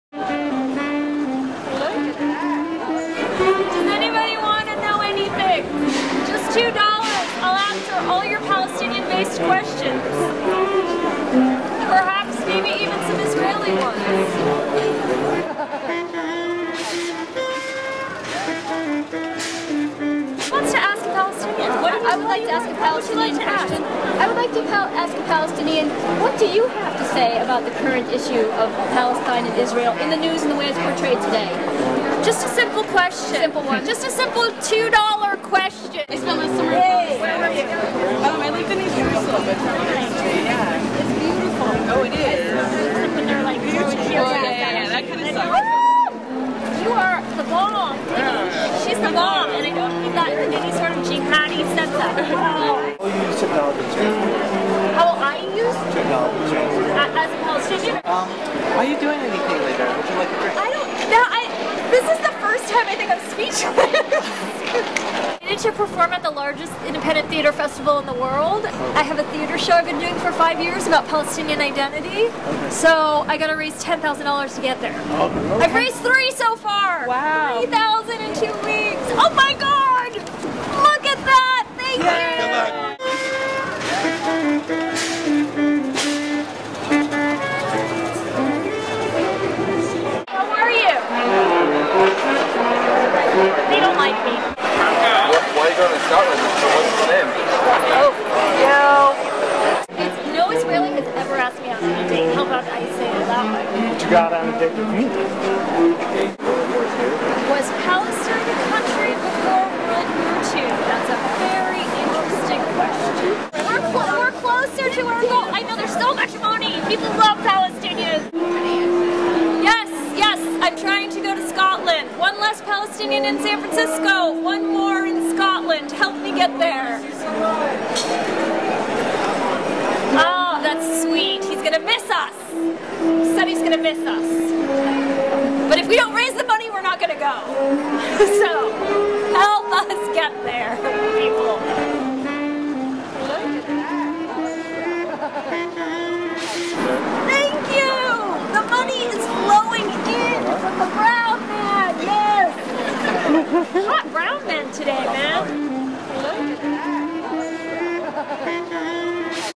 ASK A PALESTINIAN On the Streets of SF
In order to get her theatre show to Scotland, Jennifer takes to the streets of San Francisco with her ASK A PALESTINIAN sign to raise money from strangers for her Kickstarter Watch what hilarity unfolds.